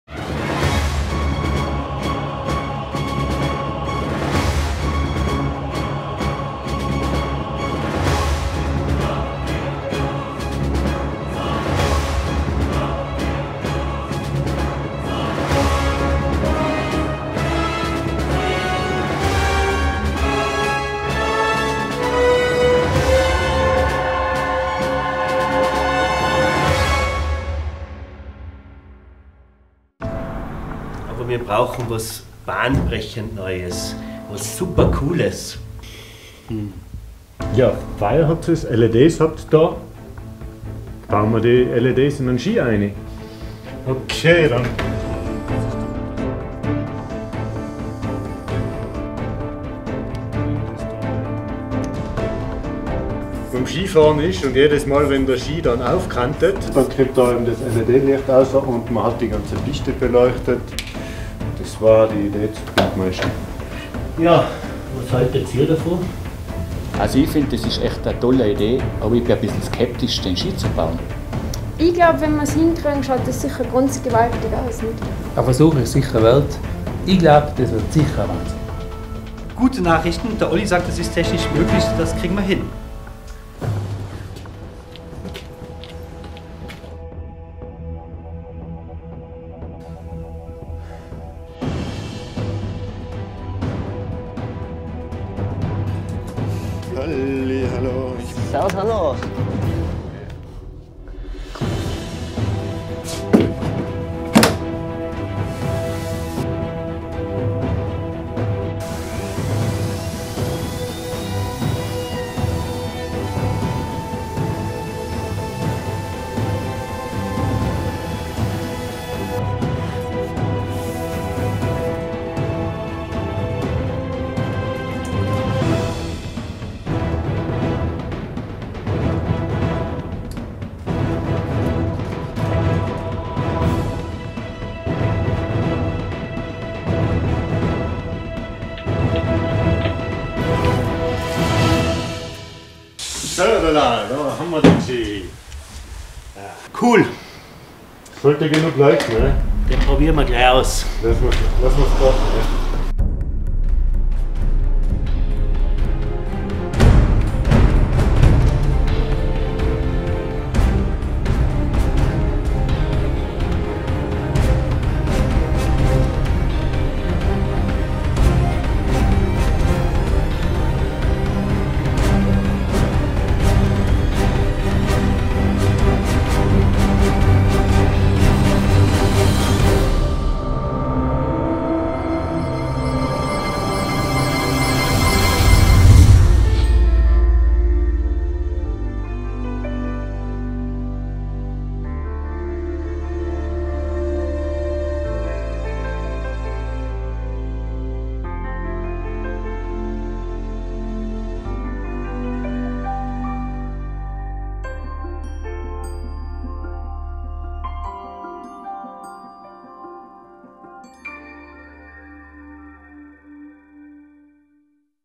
Glow Ski - every monday at the skishow "FIRE ON SNOW"
HOT...HOT...HOT... the brandnew "GlowSki"... check it out - every monday at the skishow "Fire on Snow" in Tux-Finkenberg